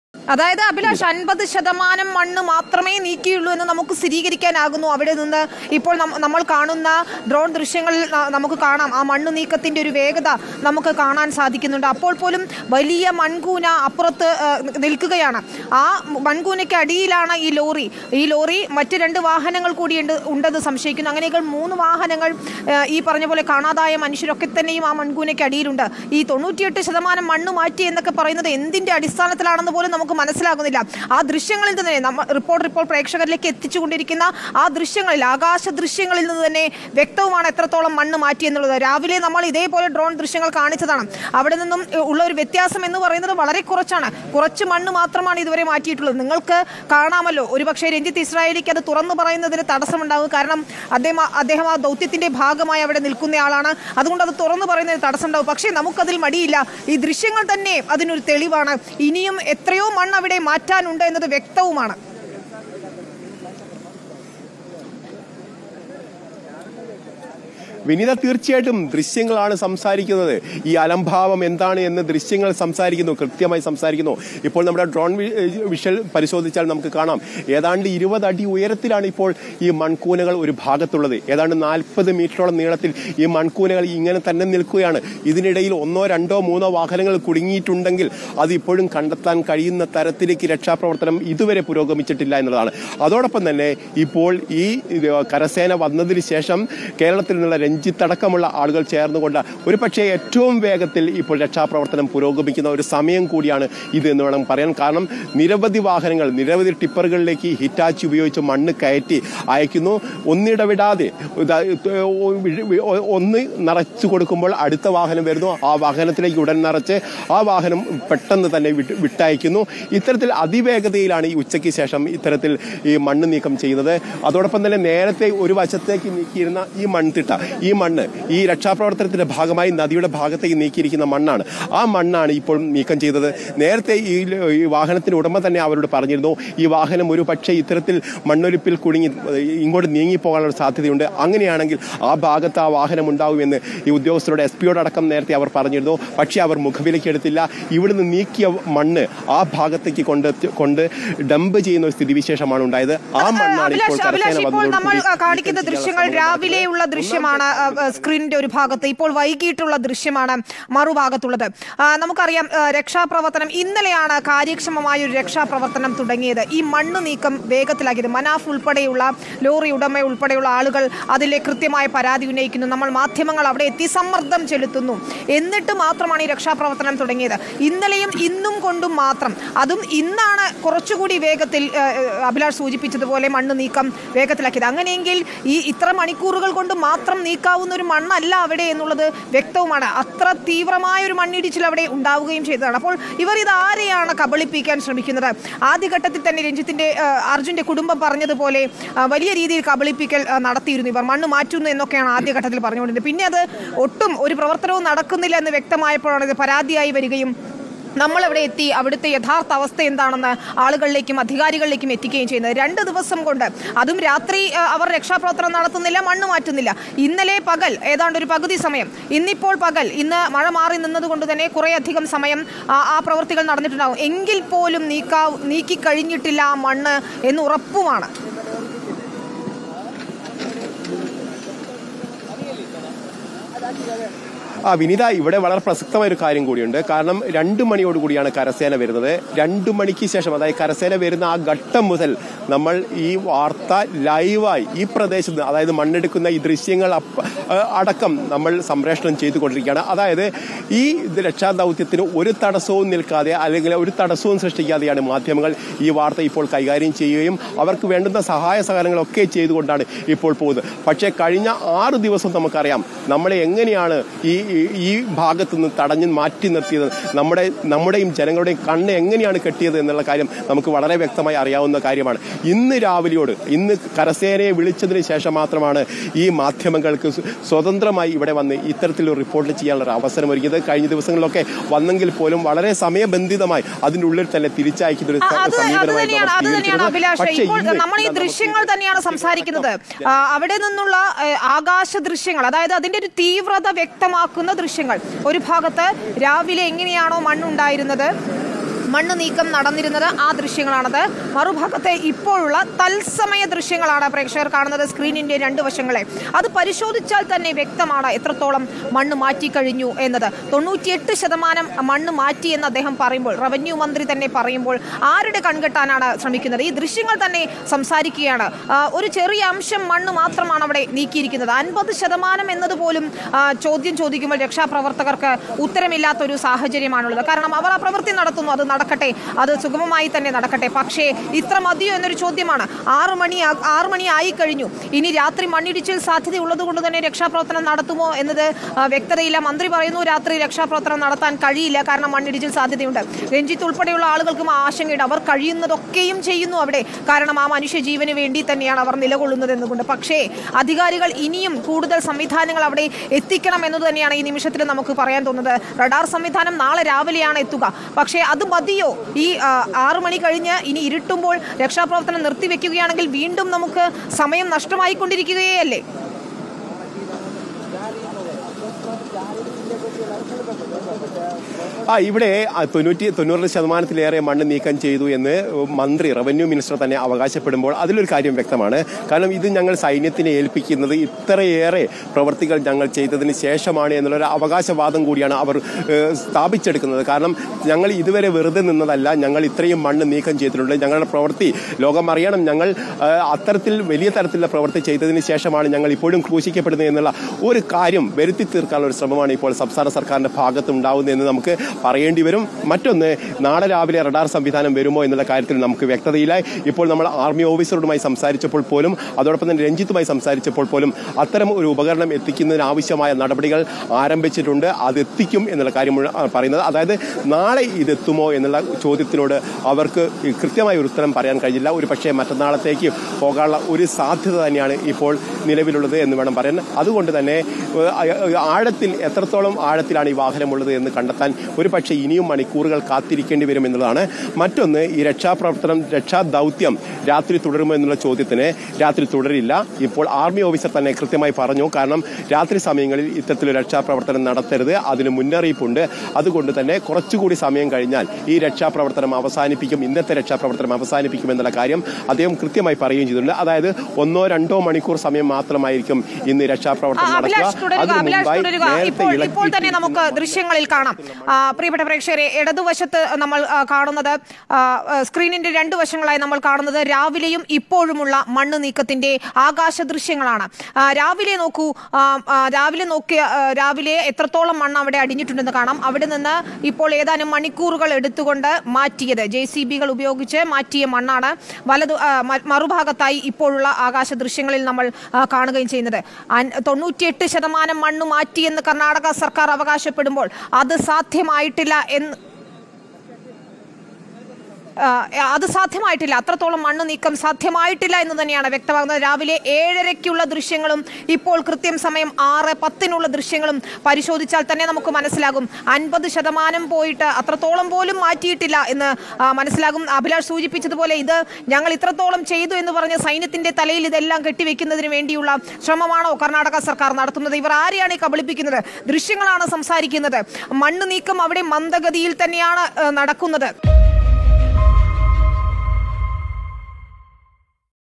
രാത്രി മണ്ണിടിച്ചിൽ സാധ്യത ഉള്ളതുകൊണ്ട് രക്ഷാപ്രവർത്തനം സാധ്യമല്ല...50 ശതമാനം പോലും മണ്ണ്.
'രാത്രി മണ്ണിടിച്ചിൽ സാധ്യത ഉള്ളതുകൊണ്ട് രക്ഷാപ്രവർത്തനം സാധ്യമല്ല...50 ശതമാനം പോലും മണ്ണ് മാറ്റിയിട്ടില്ലെന്ന് ദൃശ്യങ്ങളിൽ വ്യക്തമാണ്'; രാവിലെത്തെയും വൈകുനേരത്തേയും ഡ്രോൺ വിഷ്വൽസ് പരിശോധിച്ച് റിപ്പോർട്ടർ